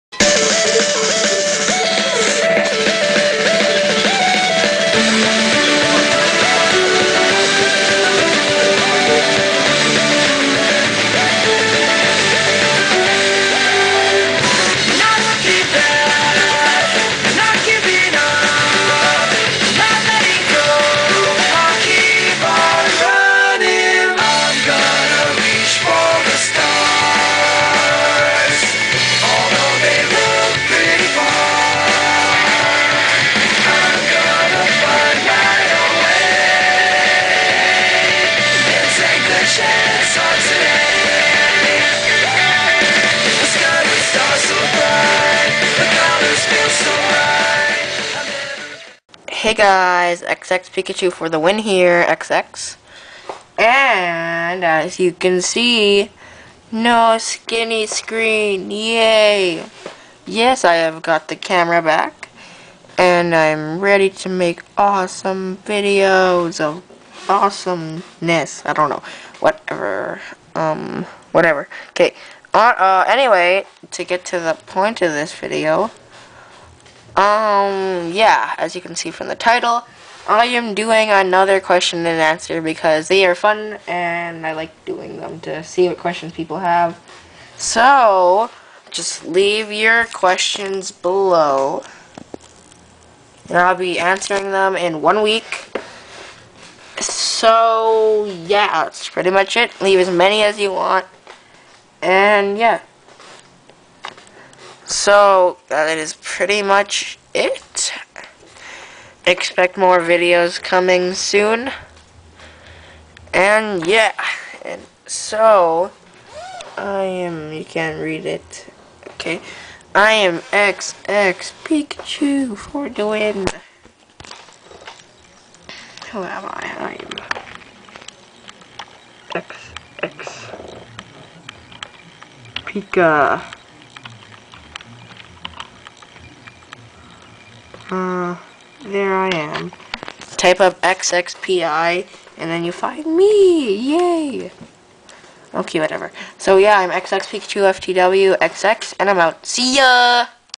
(#109) 2nd Question & Answer
Hey guys in this video I talk about doing another Q&A so leave your questions below and I'll answer them in 1 week so next saturday =P